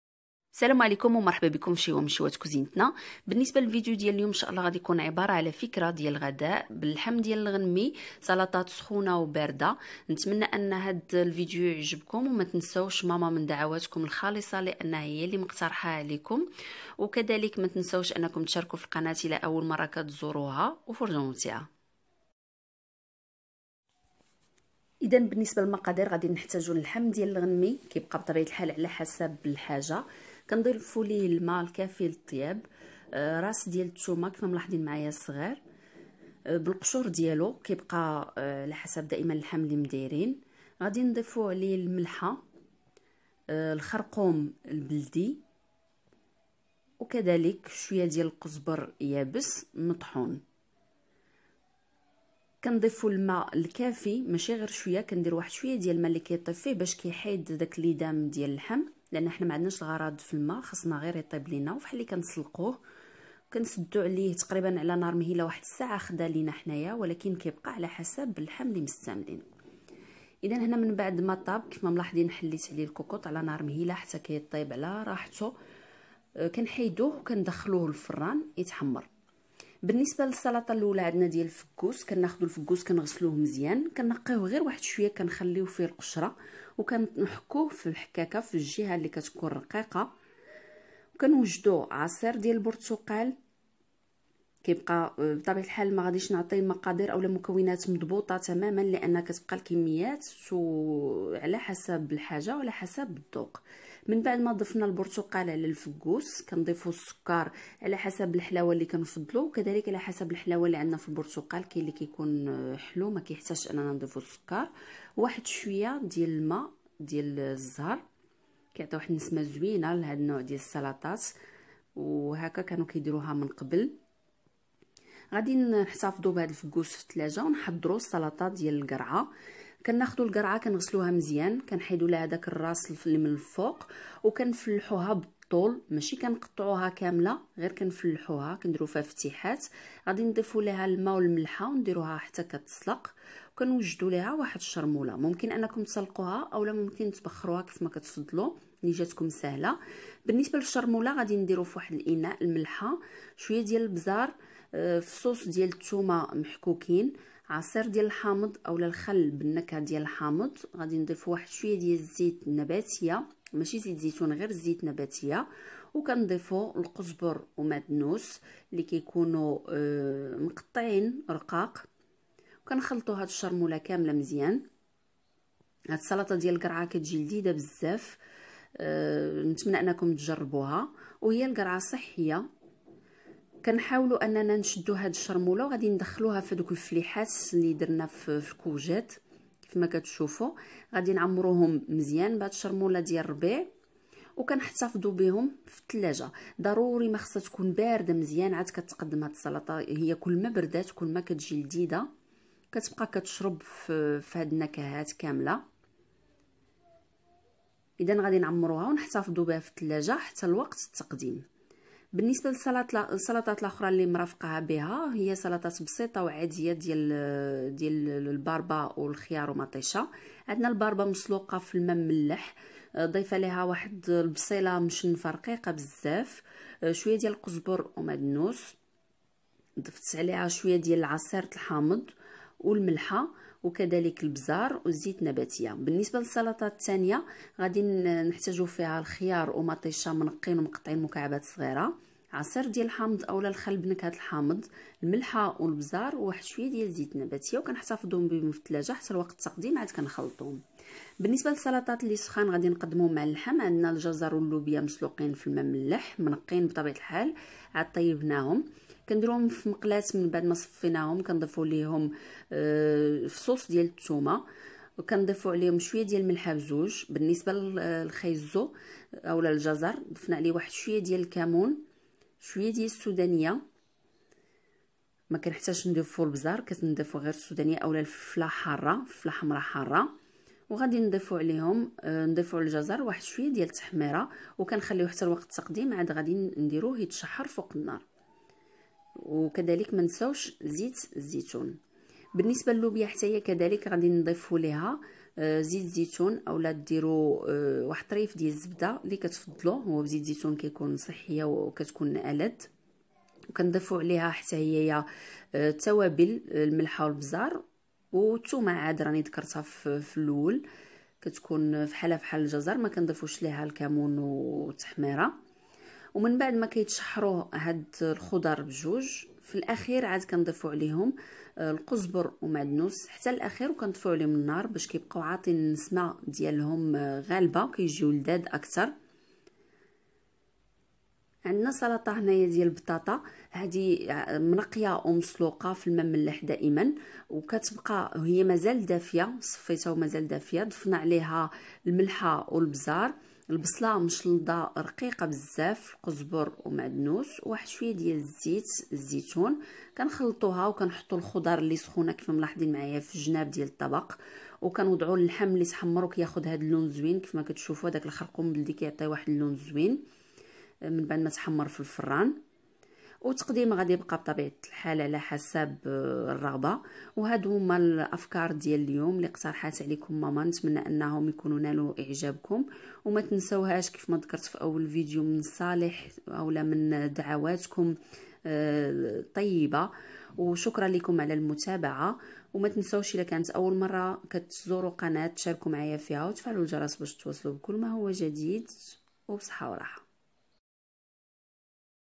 عيد_الأضحى فكرة غذاء و سلطات ساخنة و باردة
مروركم الطيب و تعليقكم المشجع
المقادير:
لحم غنمي و ملح و كركم و قزبرة يابسة مطحونة و ماء كافي

سلطة الفقوس
فقوس مبشور و عصير البرتقال و سكر حسب الذوق و القليل من ماء الزهر

سلطة الخيار و الطماطم
طماطم و خيار منقيين و مقطعين مكعبات صغيرة و ملح و فلفل اسود و زيت نباثية و خل او عصير الحامض

سلطة الشمندر
شمندر مسلوقة و ملح و فلفل اسود و خل او عصير الحاض و بقدونس و زيت نباثية